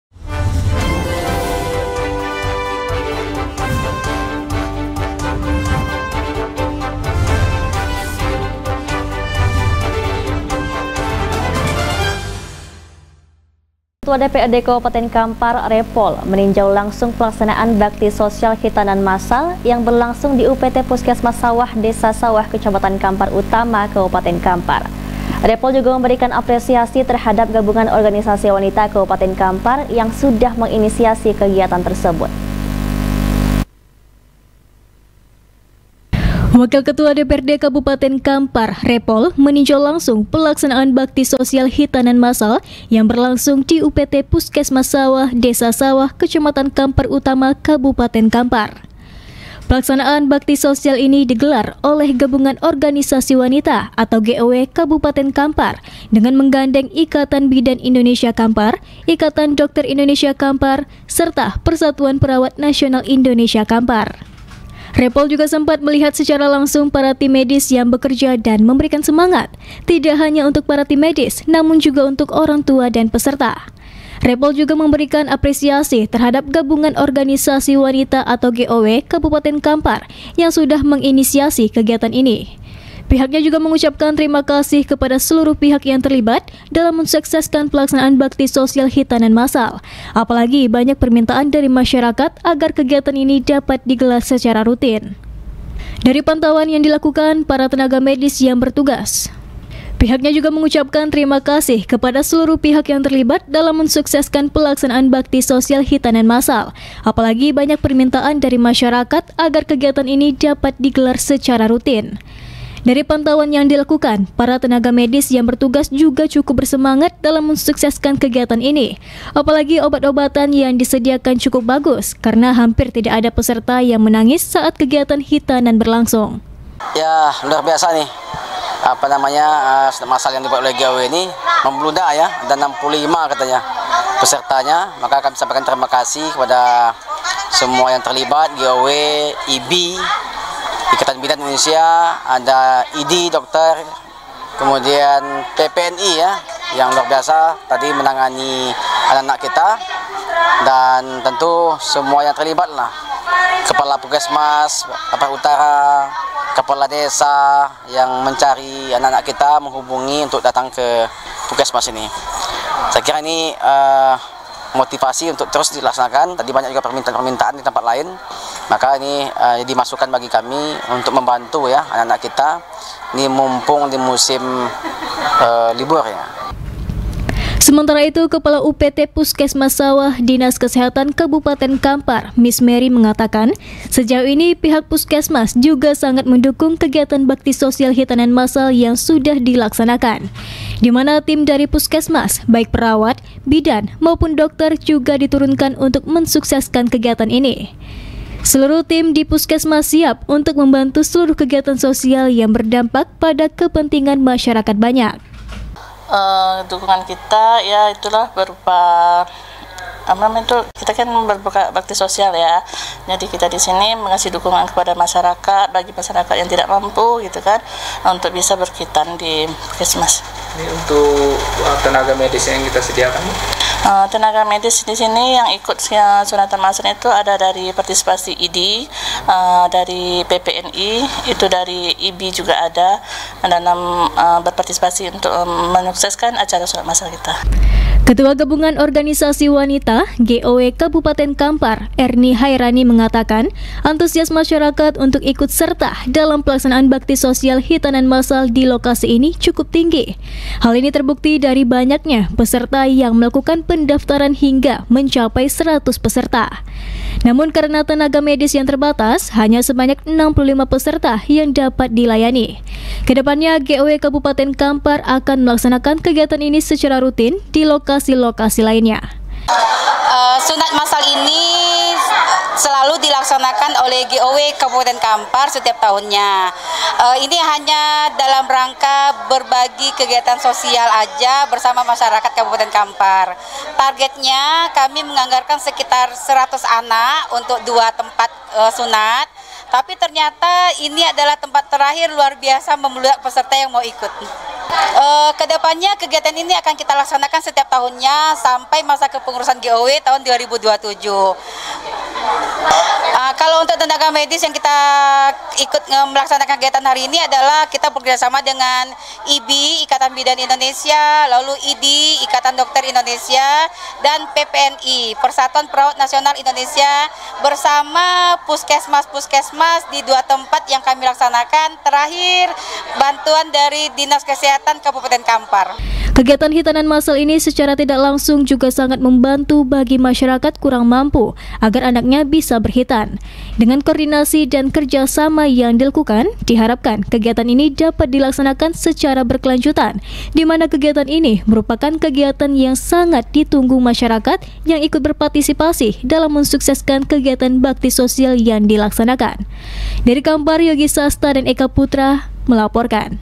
WAKIL KETUA DPRD KAMPAR REPOL TINJAU KHITANAN MASSAL DIPUSKESMAS SAWAH
Wakil Ketua DPRD Kabupaten Kampar, Repol meninjau langsung pelaksanaan Bhakti Sosial Khitanan Massal yang berlangsung di UPT Puskesmas Sawah, Desa Sawah, Kecamatan Kampar Utama Kabupaten Kampar. Repol juga memberikan apresiasi terhadap Gabungan Organisasi Wanita Kabupaten Kampar yang sudah menginisiasi kegiatan ini.